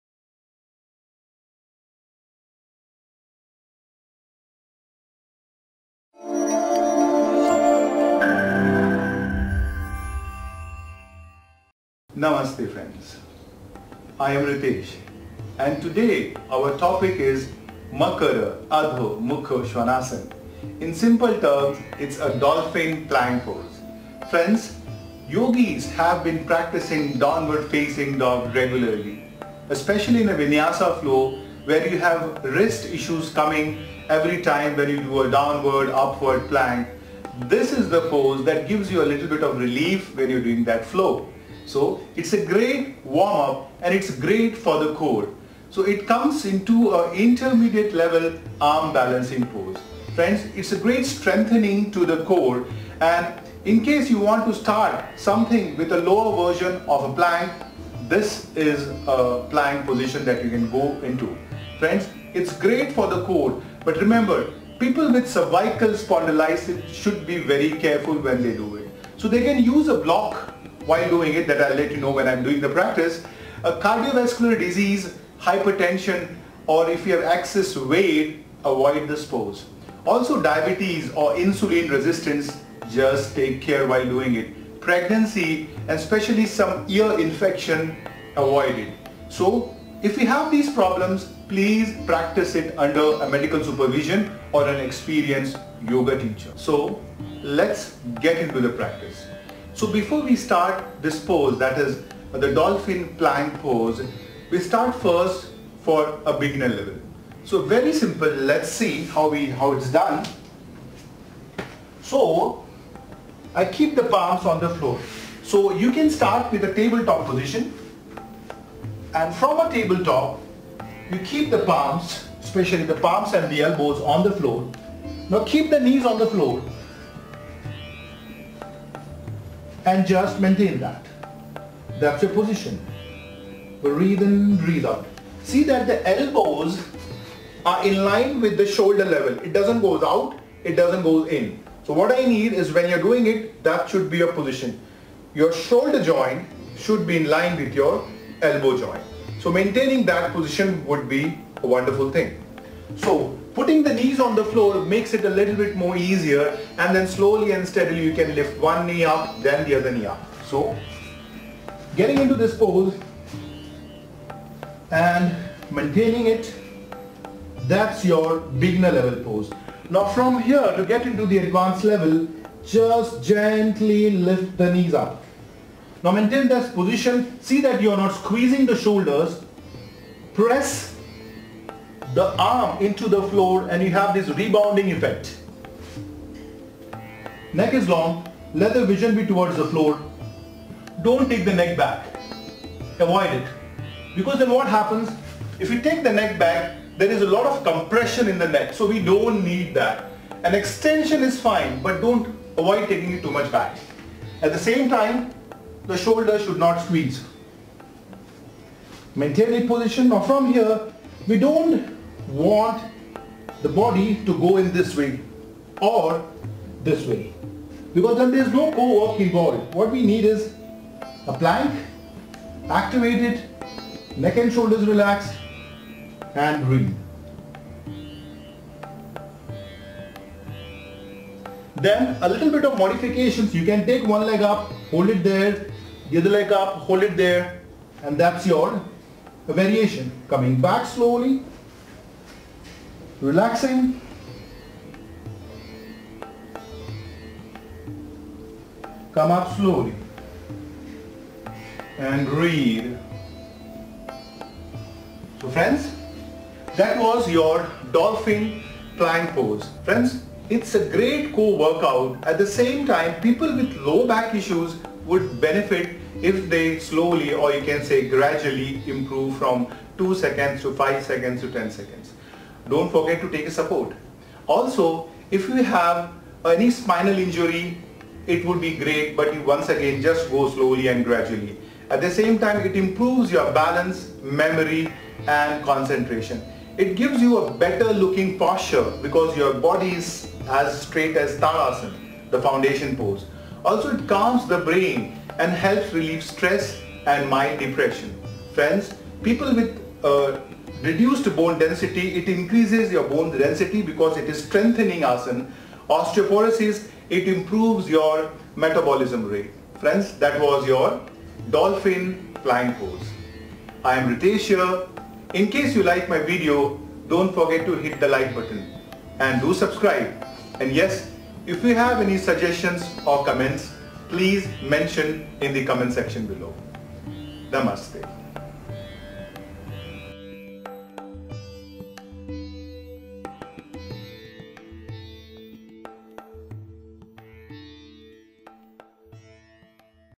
Makara Adho Mukha Svanasana (Dolphin Plank Pose) How to Do by Yogi Ritesh- Siddhi Yoga
LEARN How to do Makara Adho Mukha Svanasana (Dolphin Plank Pose) properly.  Know the benefits and contraindications from Indian Yogi Ritesh.